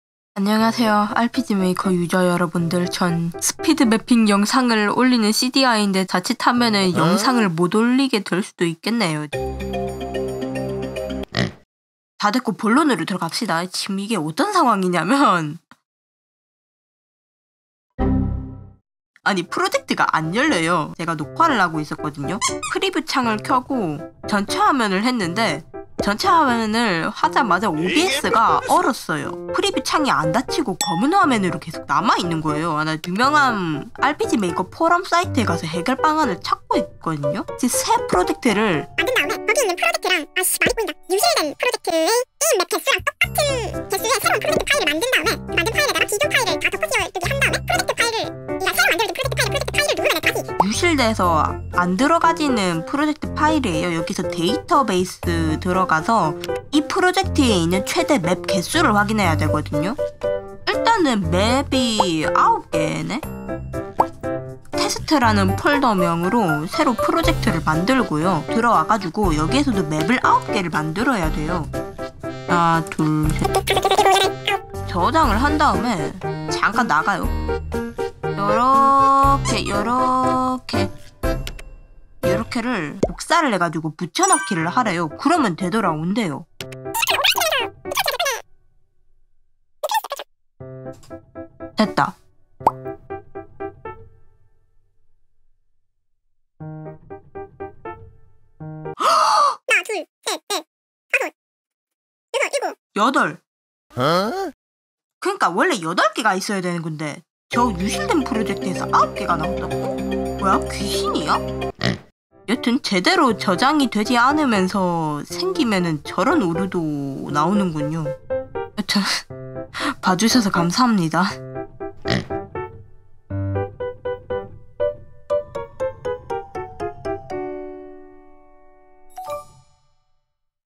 [RPG Maker XP] Unexpected file format 오류 해결법 (feat. RPG Maker Forums)
00:37 해결법 바로 ㄱㄱ

오랜만입니다. CDiA예요~
최근에 생긴 현상은 아니고 1달 전에 생겼던 현상인데 개강하고 3주동안 바빠서 영상 만들 시간이 없었네요. 아마 중간고사 전까지 계속 바쁠 것 같습니다. 더 바빠지기 전에 생존신고겸 하나라도 올리고 갑니다 (ㅎㅎㅎㅎ...)

해결법 원문은 RPG Maker Forums 사이트의 게시글 중 Andar 유저가 달은 댓글(#8)에 있습니다.

[Credits]
1. Video Source Support

2. BGM